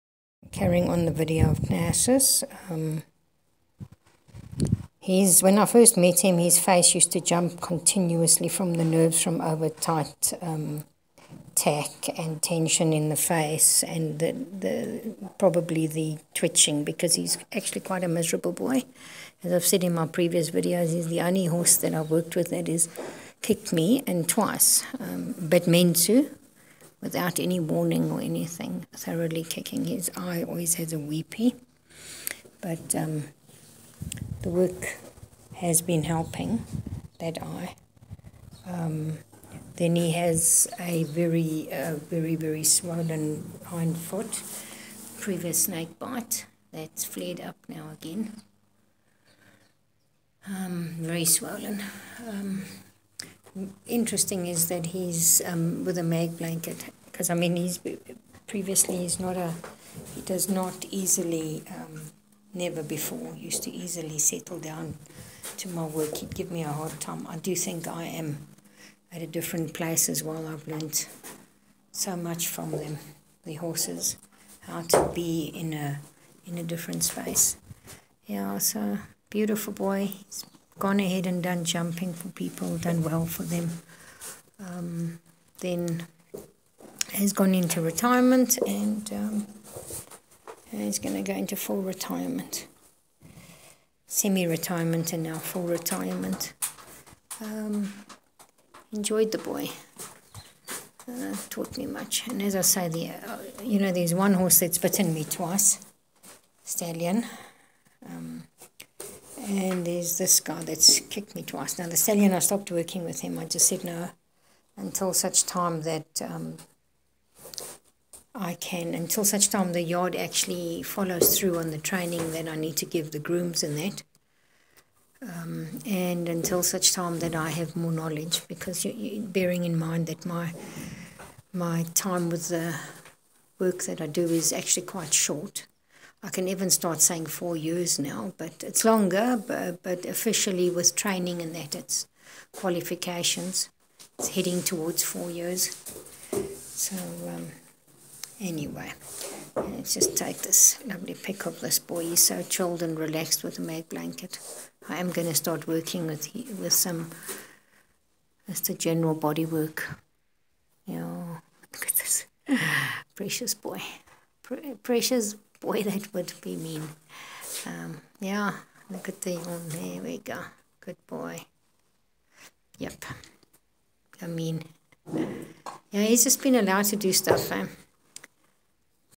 The horse Knersus into retirement Horse Massage therapy
Knersus is one of my long term regular clients. 18 months. I met him when he was competing at show jumping lower levels. He was quite the difficult horse in hand and in stable and even under saddle. His own enlisted me to come do a couple of sessions due to displaying discomfort in his work. He then went on to be treated by a physiotherapist a number of times for some deep therapy. Nice improvement, I then continued with maintenance body work, ie massage and release therapy, later went on to do some dynamic body work, i.e. in-hand mobility therapy and then found that he hated being lunged, was blessed to work with him and his rider to re-school him to accept lunging as the graceful exercise it is supposed to be. He went from a 'scary' rearing, chopping, kicking frenZied horse to a quiet focussed thoughtful partner in the lunge sessions. Also in the video I make a comment about the facial nerves constantly jumping. If one looks back over older videos